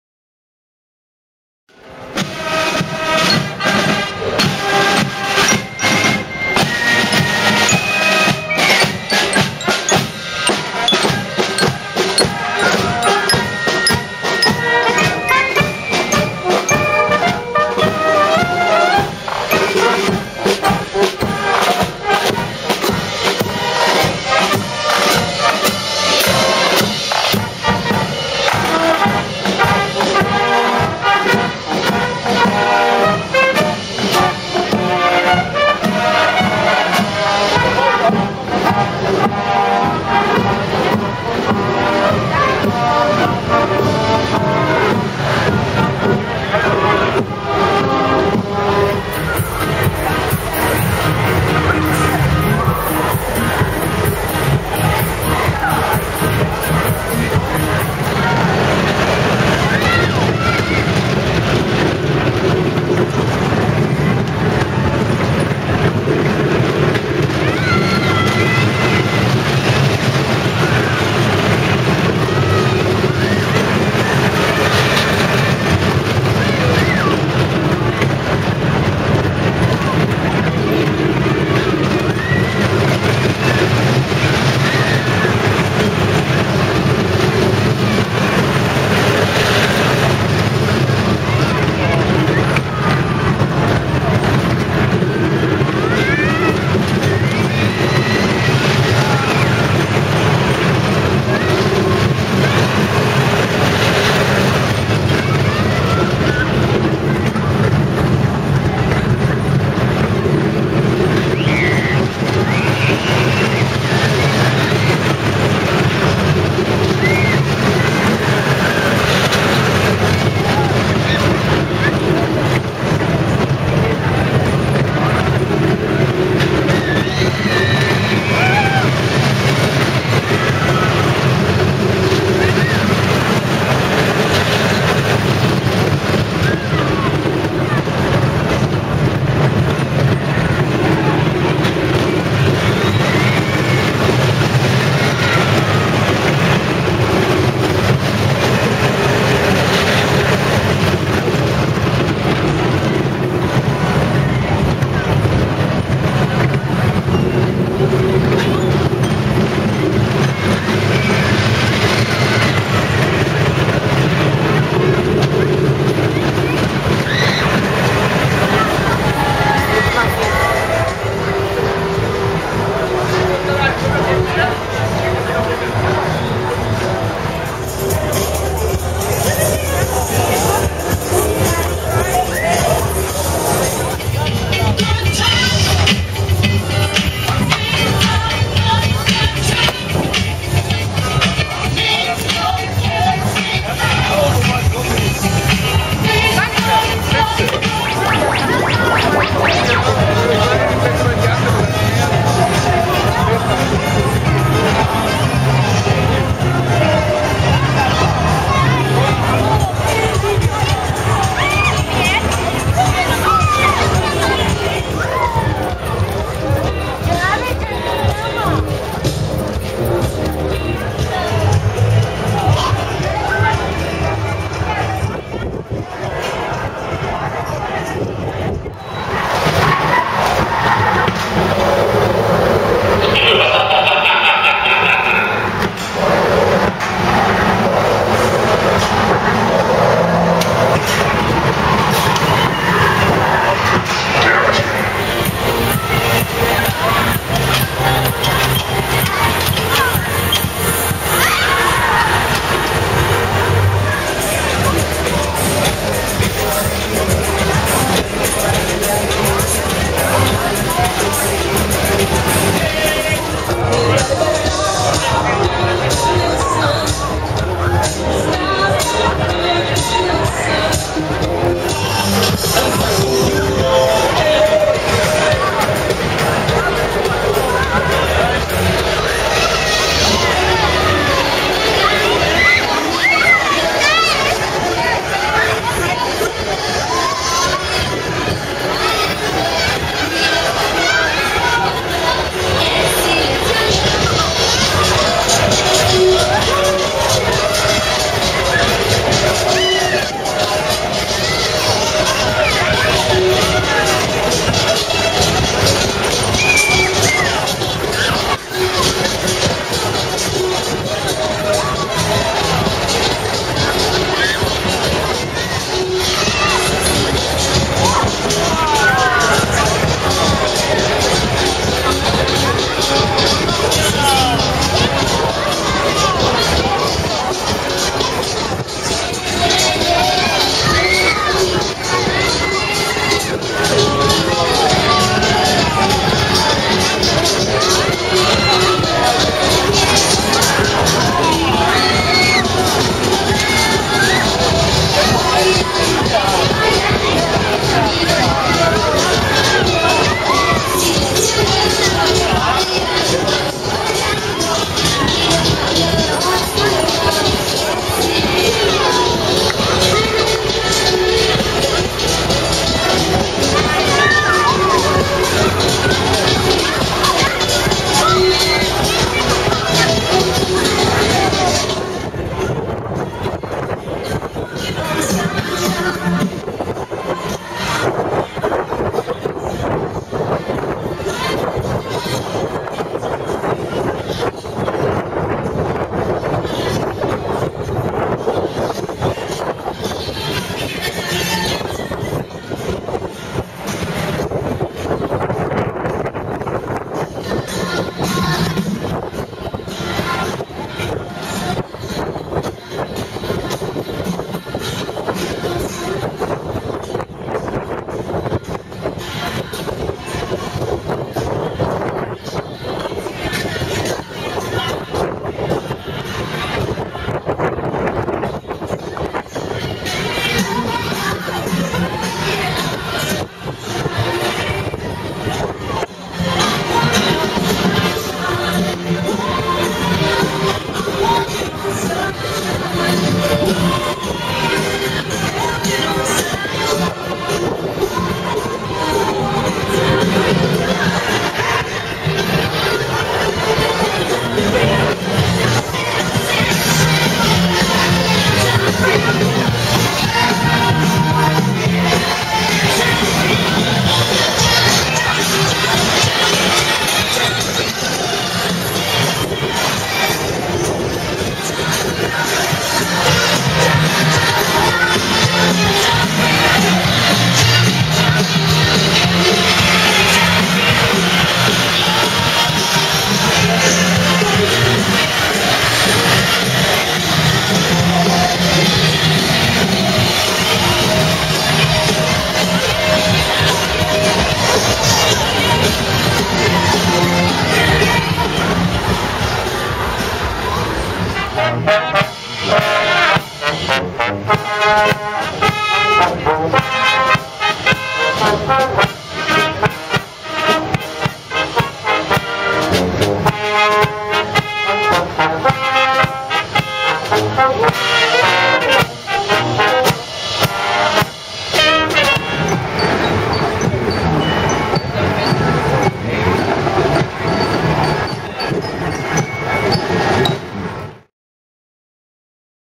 Düsseldorf- Derendorf Schützenkirmes 2013 in Action
Stadtteil Schützenfest mit Kirmes
Allround (Skyline) - Brock
Drachen (Miniachterbahn) - Janetzki
Die verrückte Farm - Cornelius
Spukschloss - Hansla
Shaker (Scheibenwischer) - Venekamp
Autoscooter,Huckelpiste,Kinderzug,Bungee-Trampolin,Simulator,Angry Bird Game